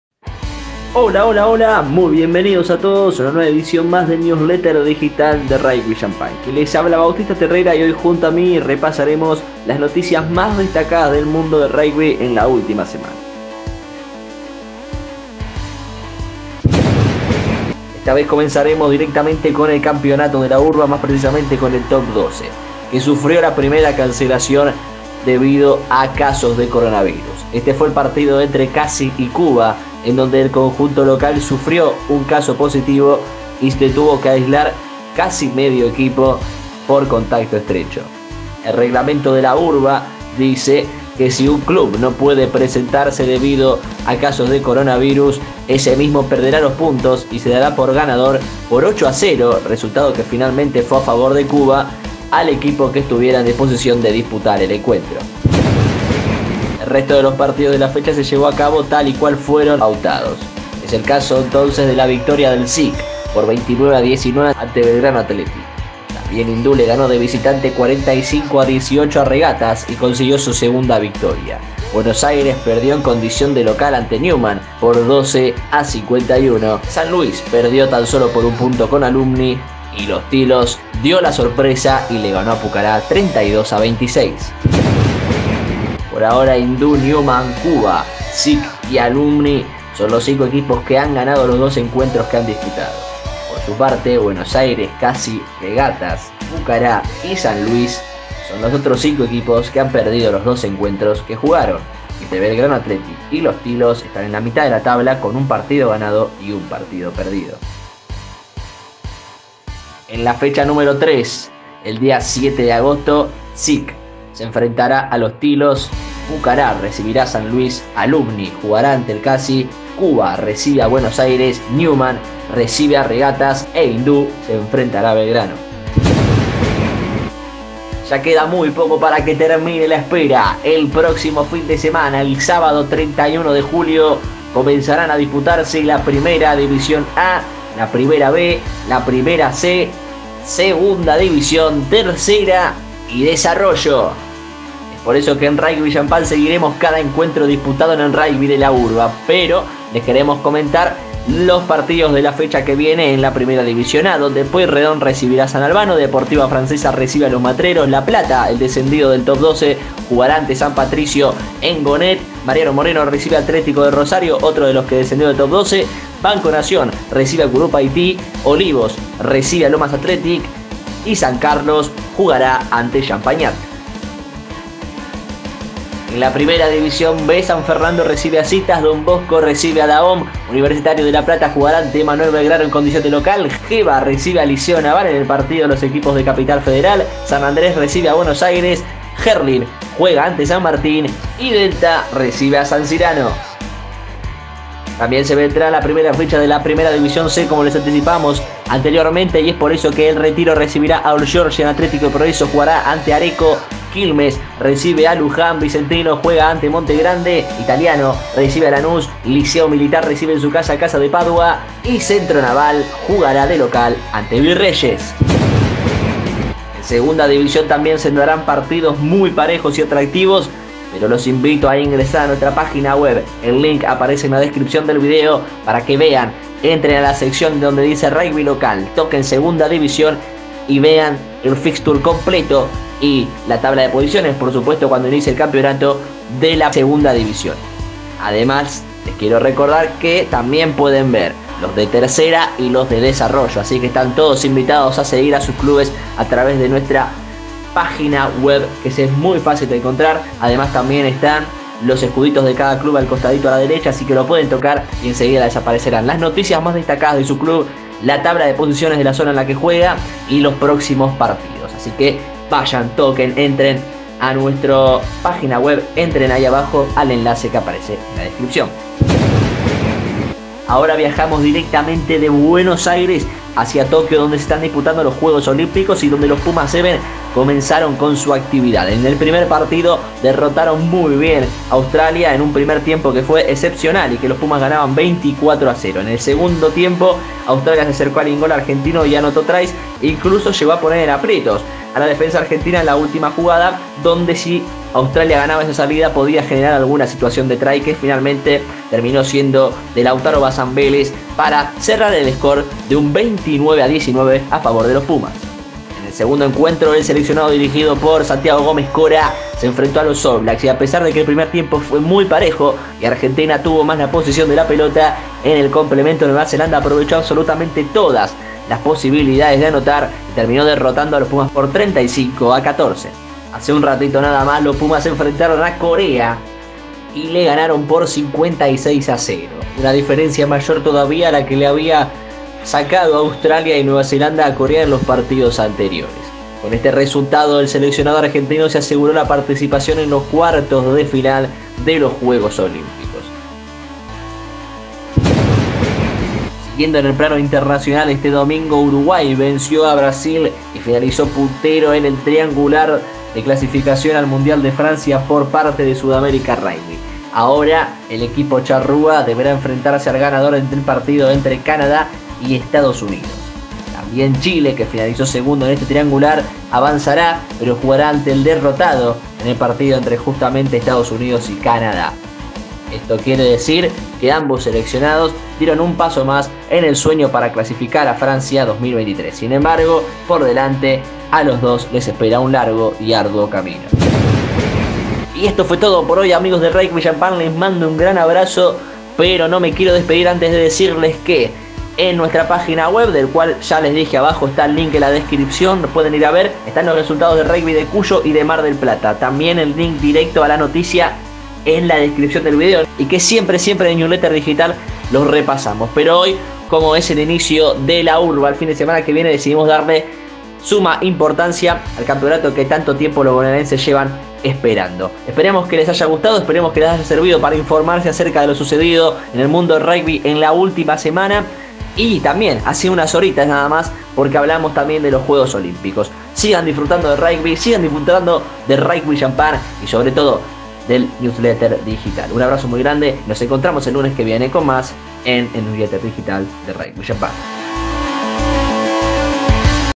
Newsletter Digital | ¡LOS PUMAS 7's ESTÁN EN CUARTOS! LO QUE SE VIENE EN EL CAMPEONATO URBA y más...
Bienvenidos al canal de YouTube de Rugby Champagne.
Este es el 25º Newsletter Digital, donde encontrarás el resumen de las noticias más importantes del rugby en la última semana.